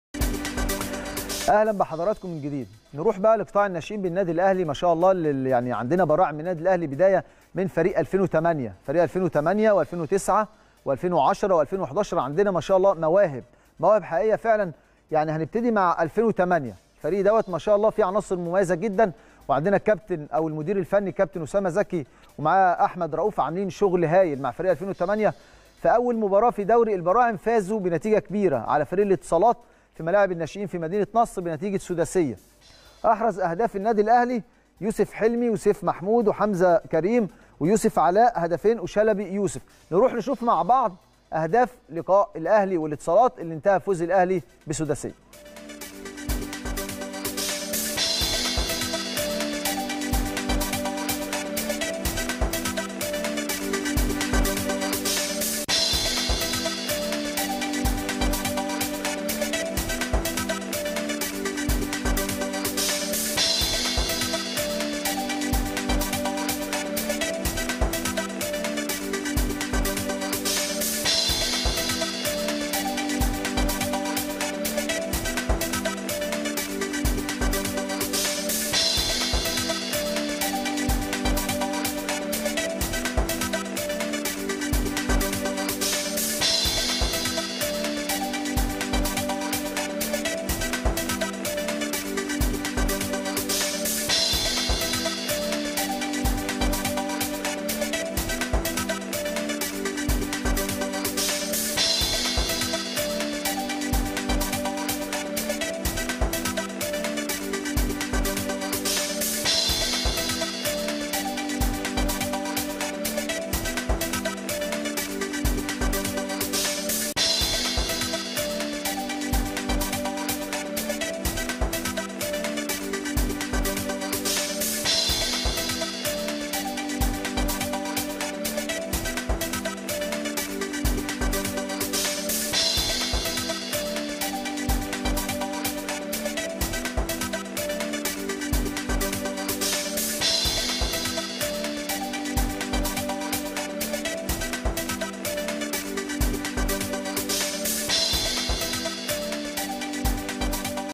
شاهد.. أهداف فريق الأهلي 2008 في مرمى الاتصالات 6-0 | الأشبال
مهرجان أكاديميات الأهلي بفرع مركز شباب الجزيرة | الأشبال